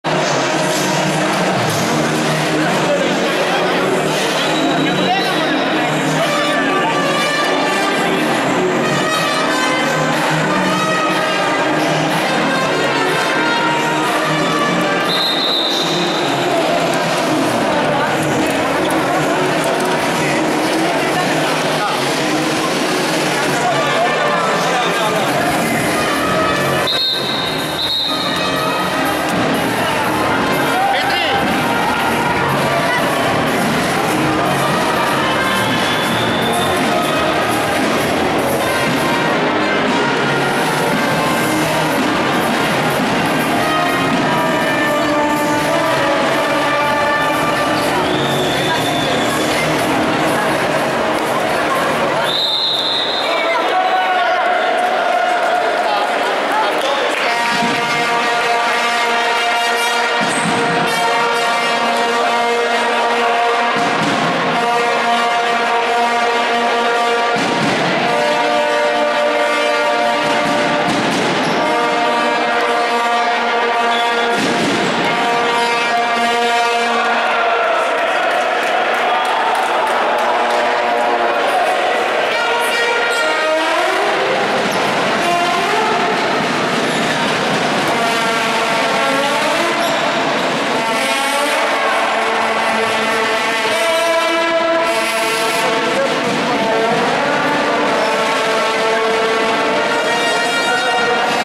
Α.Ο Φαίακας Corfu Handball-Έναρξη 2ου Ημιχρόνου & ..πρίν',5-5-2019,Βίντεο 11ο
Σ' αυτή ημίχρονο του παιγνιδιού με σκόρ 5-7 για τους Βετεράνους,επιμελής Θέαση όλων των Παικτών κοντά στους Πάγκους & έναρξη 2ου ημιχρόνου(2Οάλεπτης διάρκειας).!Halftime of Faiakas Corfu Handball & 5-7 score for Veterans(white bdressing) & a ..careful view of All Them.!Start of 2nd Half(20 Minutes Duration).!